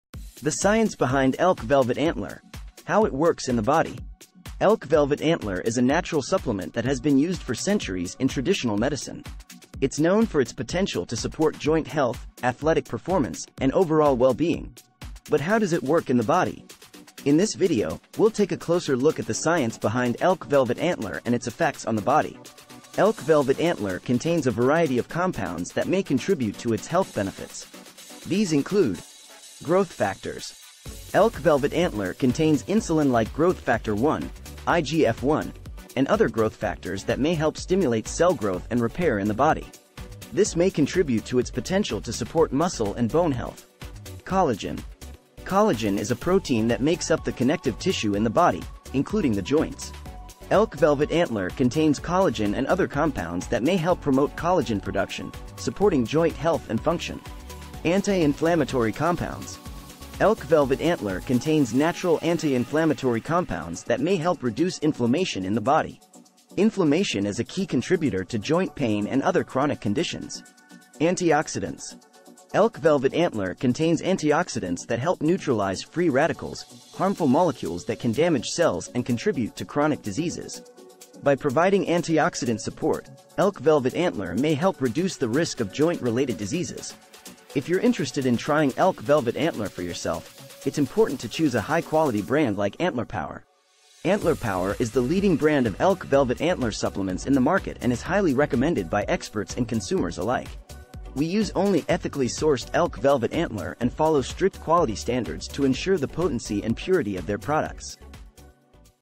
Elk Velvet Antler: How It Works in Your Body and Boosts Your Health
In this video, we dive deep into the science behind elk velvet antler and how it works in the body. Elk velvet antler has been used for centuries in traditional medicine, and in recent years, it has gained popularity as a natural supplement for various health benefits.

We'll explore the key compounds found in elk velvet antler, including insulin-like growth factor 1 (IGF-1), amino acids, and collagen, and how they work together to support joint health, boost energy levels, and improve athletic performance.

We'll also discuss the research and studies that have been conducted on elk velvet antler, and what they have found about its potential benefits. Whether you're an athlete looking to enhance your performance, or someone looking to support your overall health and wellness, this video is for you.

Join us as we uncover the fascinating science behind elk velvet antler and its impact on the body.